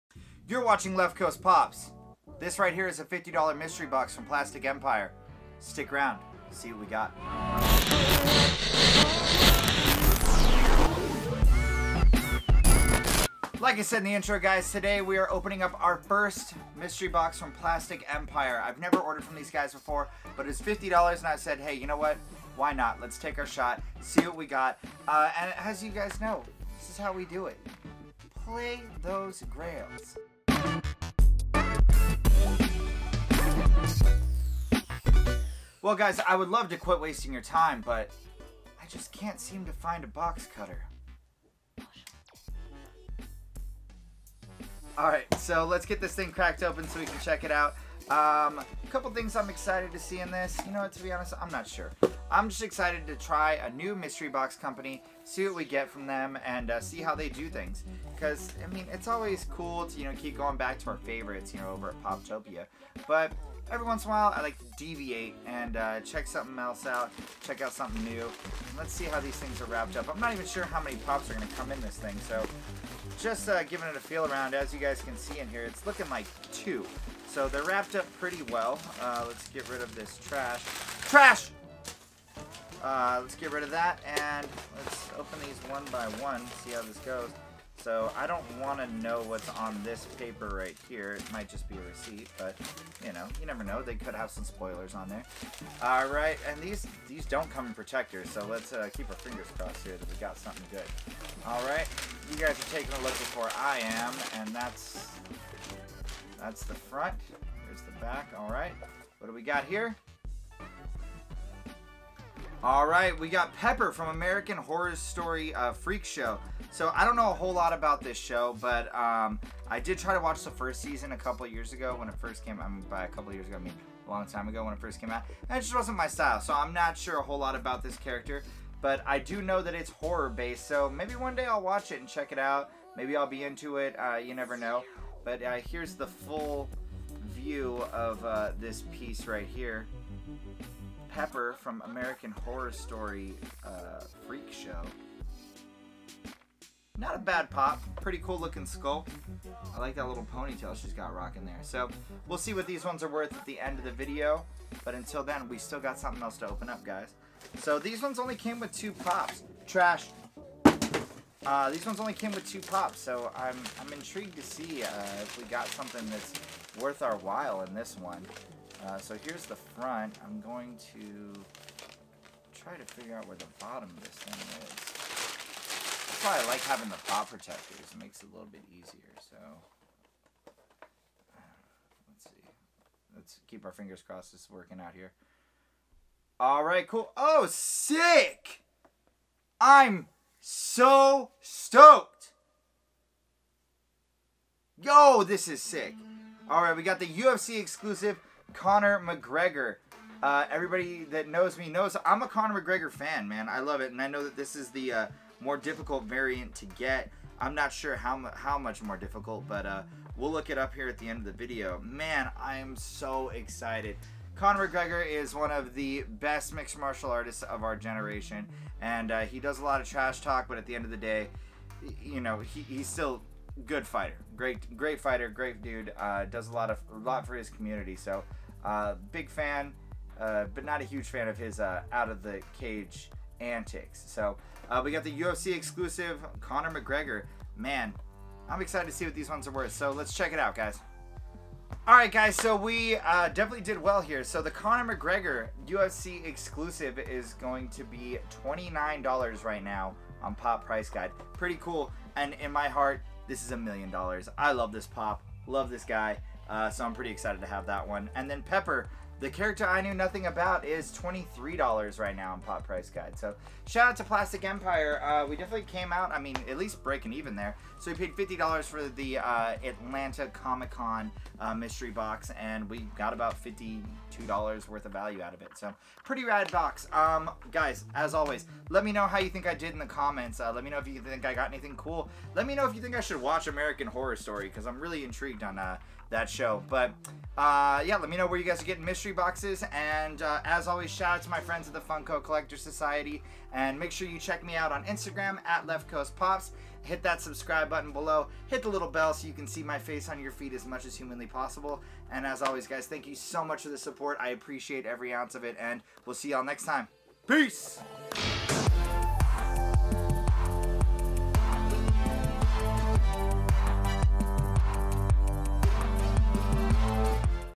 $50 PLASTIC EMPIRE MYSTERY BOX
Today we open up a $50 mystery box from Plastic Empire!